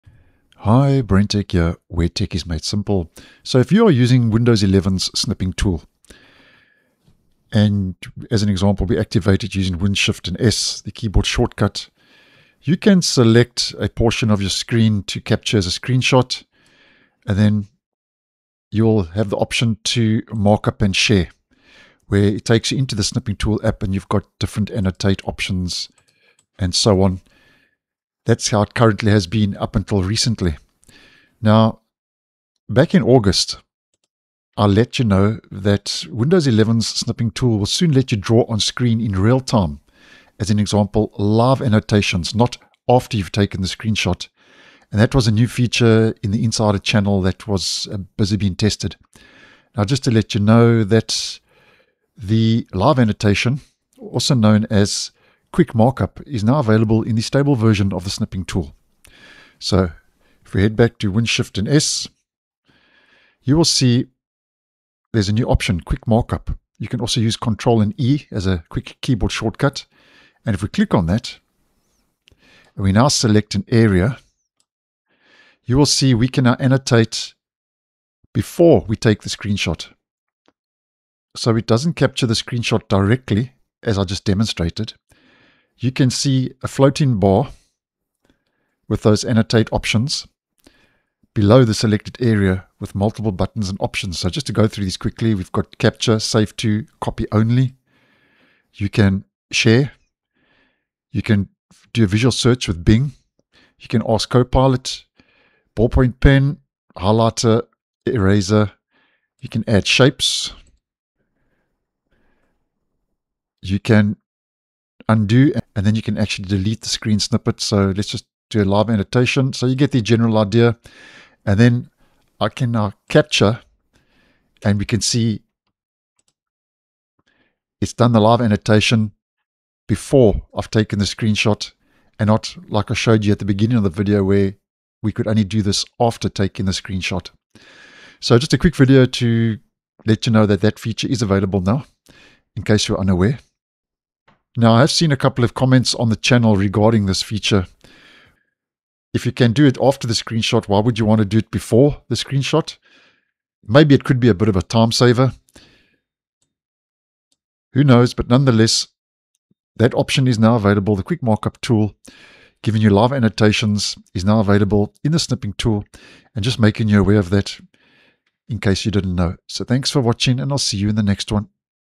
Windows 11 Snipping Tool Now Lets You Annotate on Screen: New Quick Markup!
Quick Markup now lets you draw on your screen in real time (Live Annotation).